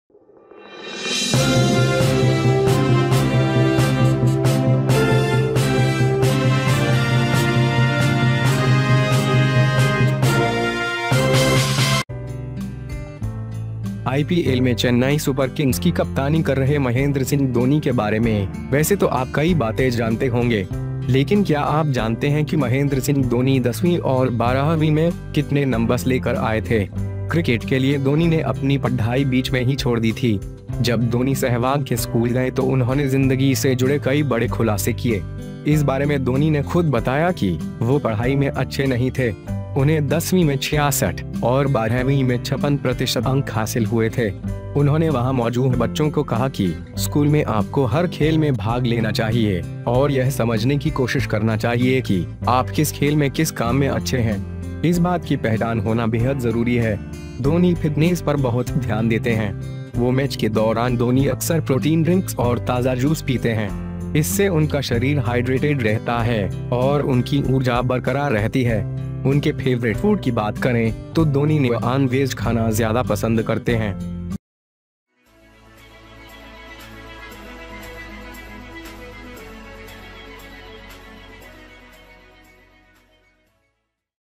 धोनी को 10वीं में मिले थे इतने मार्क्स, देखकर हैरान रह जाएंगे आप...
12वीं तक पढ़े हैं फर्राटेदार इंग्लिश बोलने वाले धोनी, 10वीं के मार्क्स जानकर हैरान रह जाएंगे...

Regarding Mahendra Singh Dhoni, who captained Chennai Super Kings in IPL, you may know many things. But do you know how many Nambas were brought by Mahendra Singh Dhoni in the 10th and 12th?
For cricket, Dhoni left his studies in the middle. When Dhoni went to Sehwag's school, he did many big disclosures related to life.
Dhoni himself said that, he was not good at studies. He got 66 percent marks in class X and 56 percent in XII. They told the children there, that in school you should participate in every game, and try to understand that in which game you are good at. It is very important to have this identity.
Dhoni gives a lot of attention to fitness. During that match, Dhoni often drinks protein drinks and fresh juices. This keeps their body hydrated, and their energy remains intact. Talk about their favorite food, Dhoni likes to eat nan wage.

HJ NEWS !!